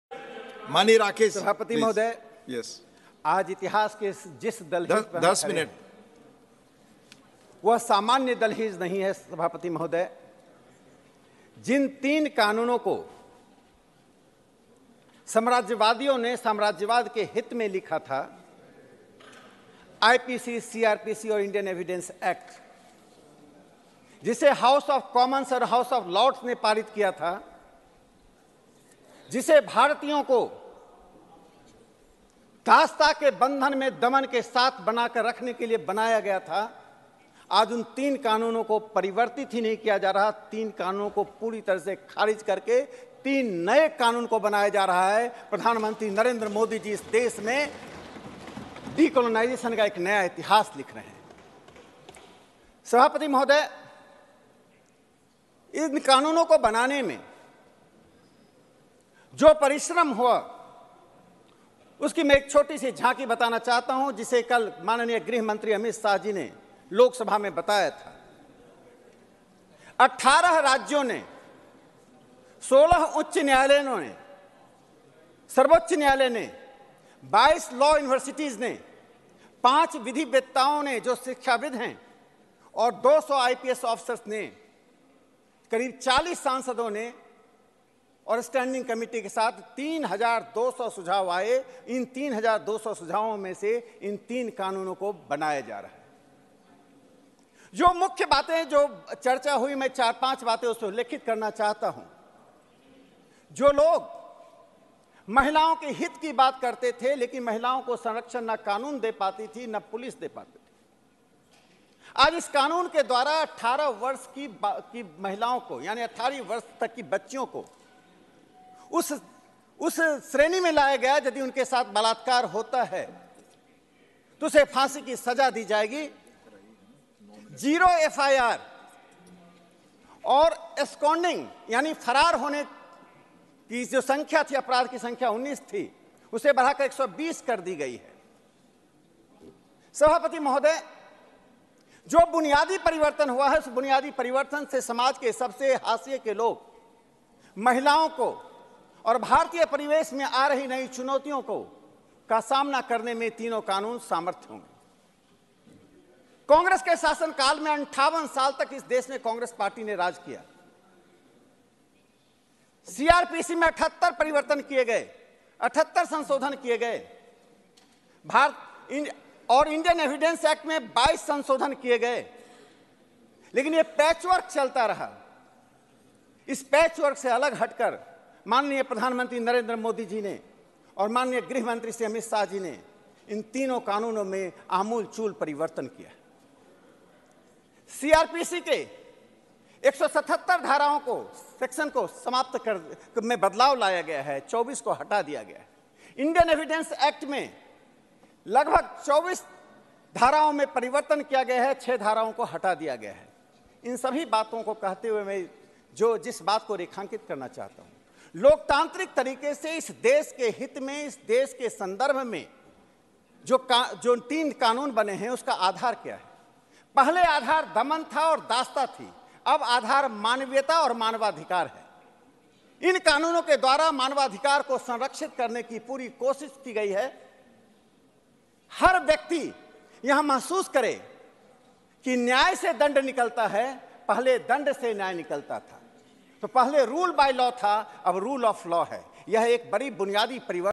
Truck Drivers Strike : Hit And Run Law पर Rakesh Sinha ने कर दी सबकी बोलती बंद! | Hindi News | N18V
Rakesh Sinha said that the three laws which were written by the imperialists in the interest of imperialism... today a new history of decolonization has been written by rejecting those three laws.

राकेश सिन्हा ने कहा की जिन तीन कानून को साम्राज्यवादियों ने साम्राज्यवाद के हित में लिखा था... आज उन तीन कानून को ख़ारिज कर के decolonization का एक नया इतिहास लिखा गया है